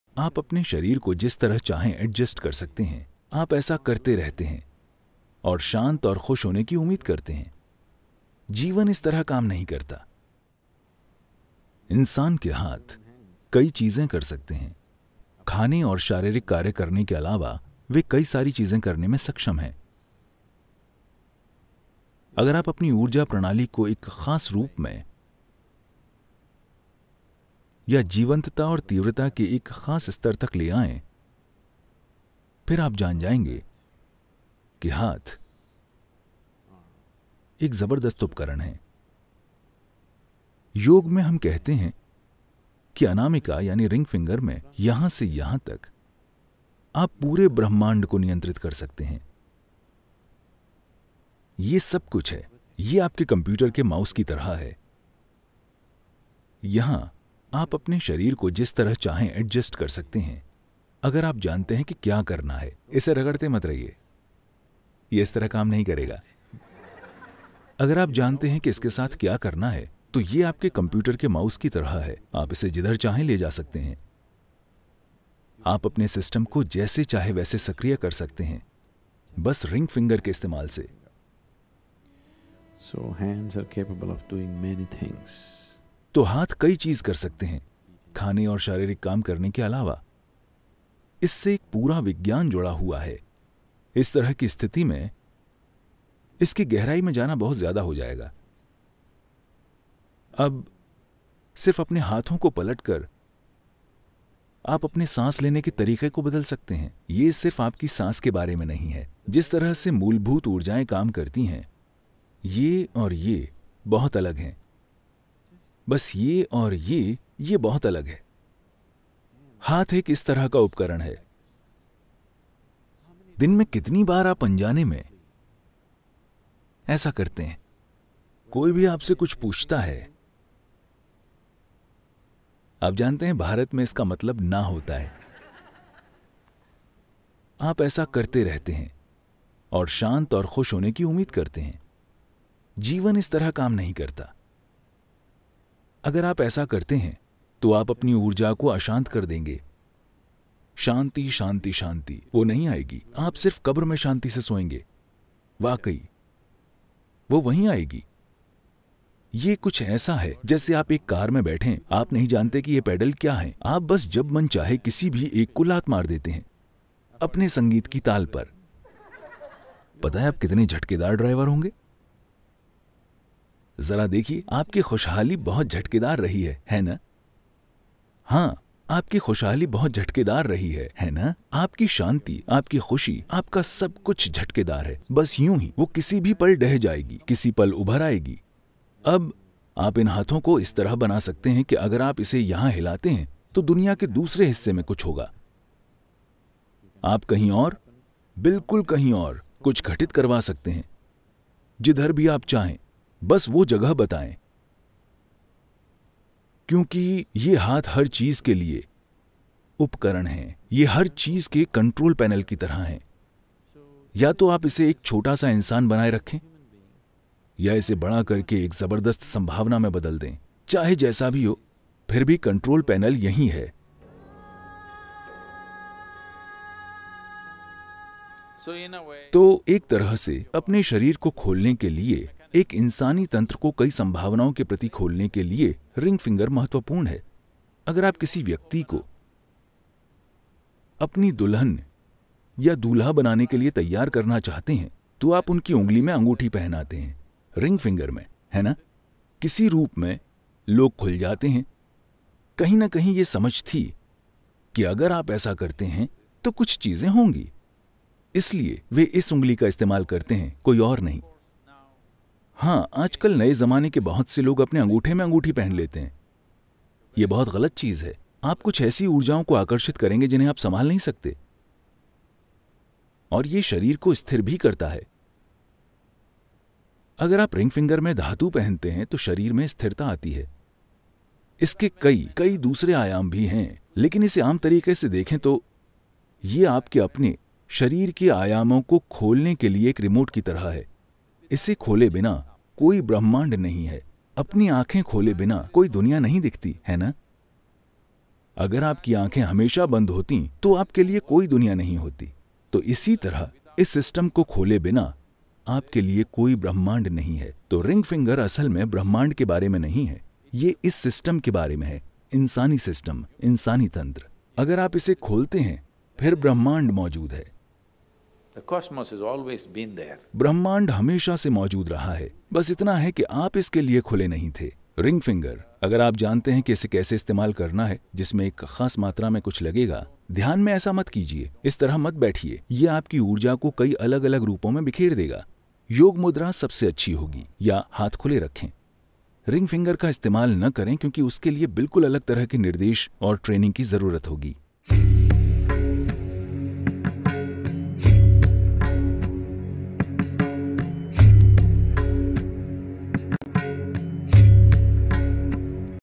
🟠Secrets of the ring finger | Sadhguru Sutra
Have you ever pondered the deeper significance behind the ring finger? In this thought-provoking video, Sadhguru unravels the mysteries and hidden meanings associated with this often-overlooked digit. The ring finger, adorned with symbols of commitment and connection, holds secrets that go beyond the tangible.

Sadhguru shares profound insights into the cultural, spiritual, and symbolic aspects of the ring finger, exploring its role in relationships, rituals, and personal growth. As you embark on this journey of understanding, you'll gain a newfound appreciation for the subtle yet powerful energies that converge at the heart of this ancient tradition.

With a focus on Hindi-speaking viewers, we present the teachings and wisdom of Sadhguru in Hindi language, making it accessible and relatable.

Please consider sharing this video with those who may benefit from it.